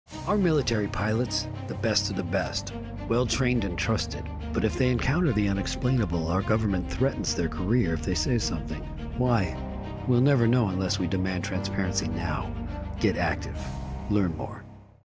U.S Military Pilots Career's Threatened after UFO Encounters
Our U.S Military Pilots are the Best of the Best, well trained and trusted; but when they encounter the unexplainable our government threatens their career if they say something.

Why?

Demand UFO/UAP Transparency NOW.